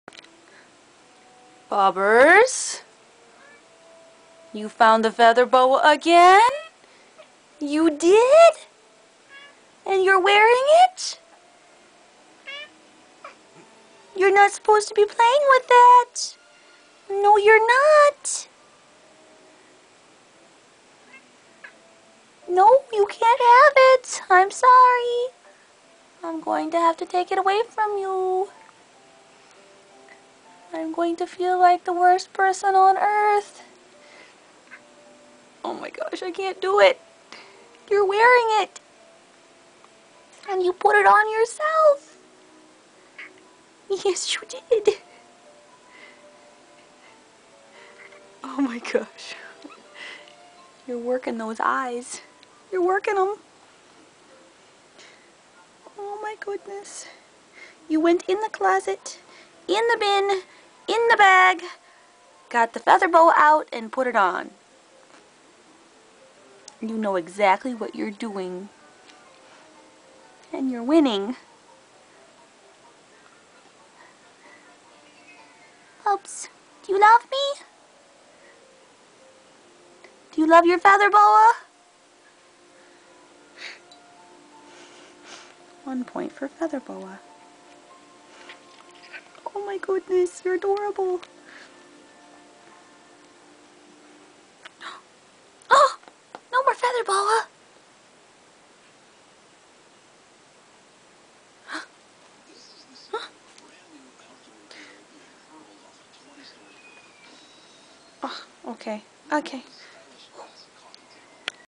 Adorable Scottish Fold kitty finds feather boa and puts in on himself
He went in the closet... in the bin.. then into the bag to get the feather boa out himself.  I found him wearing it!